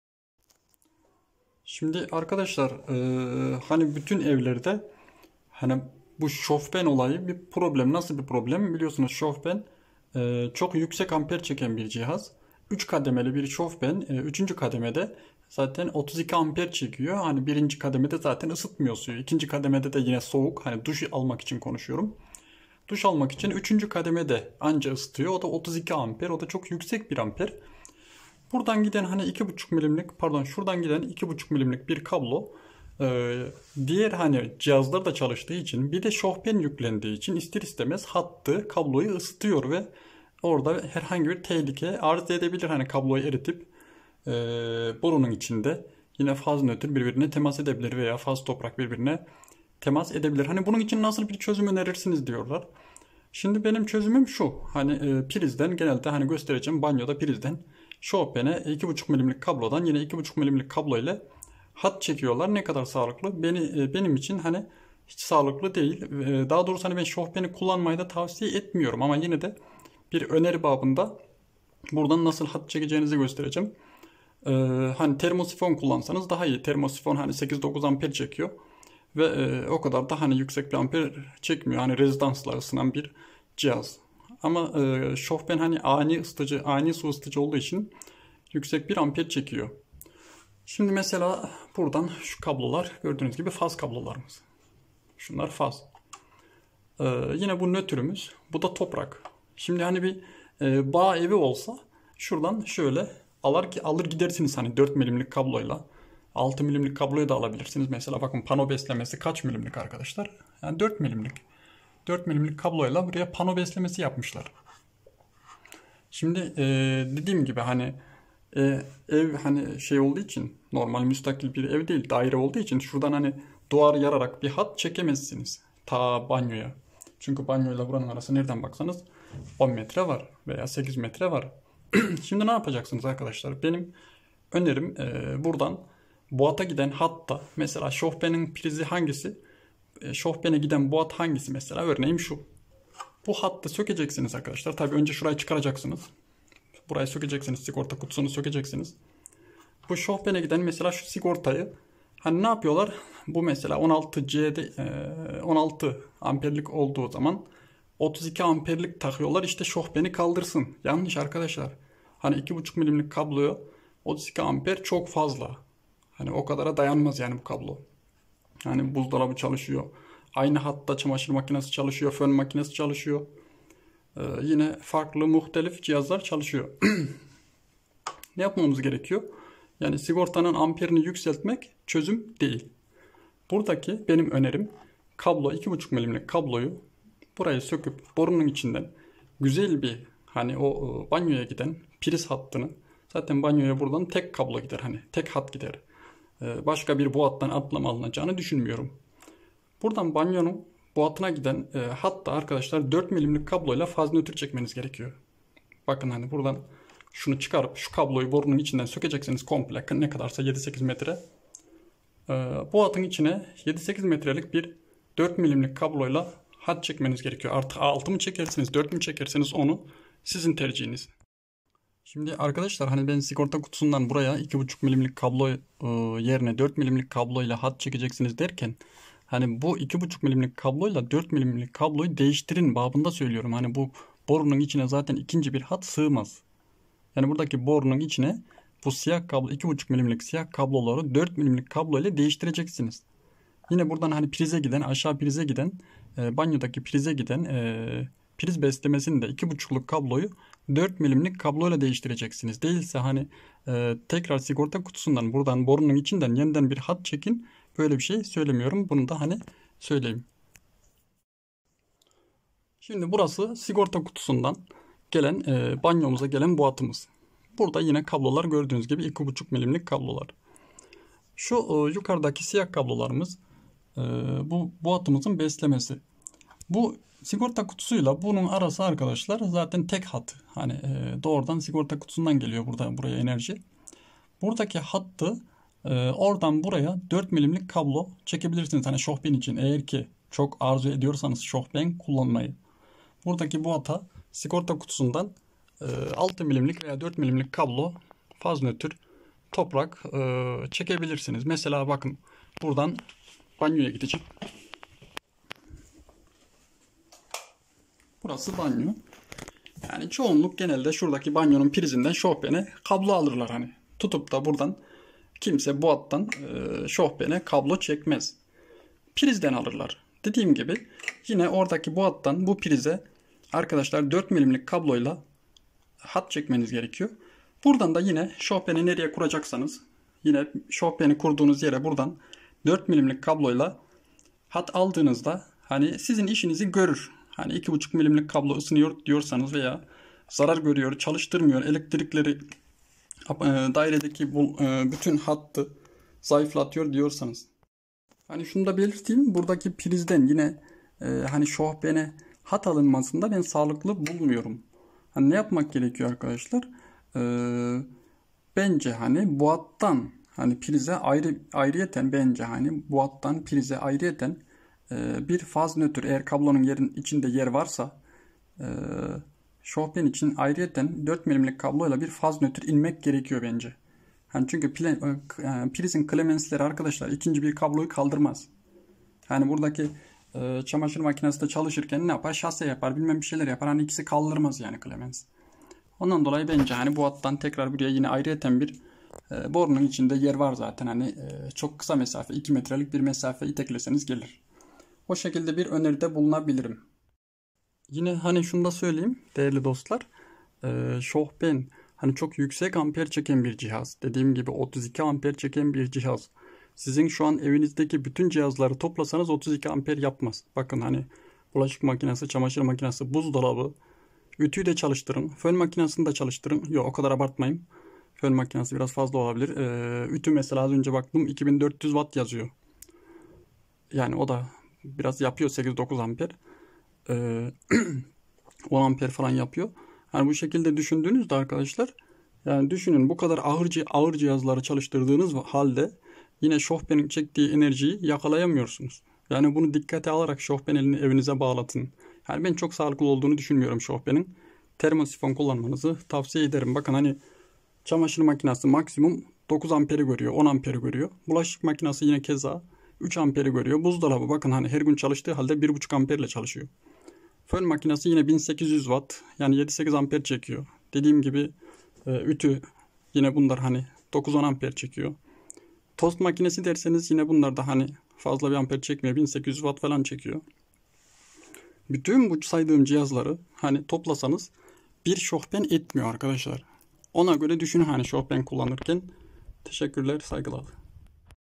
Sigorta Kutusundan Şofben için Hat çekme,şofben Elektrik kablosu nasıl çekilir.
Sigorta Kutusunda Şofben için Hat, Elektrik kablosu nasıl çekilir.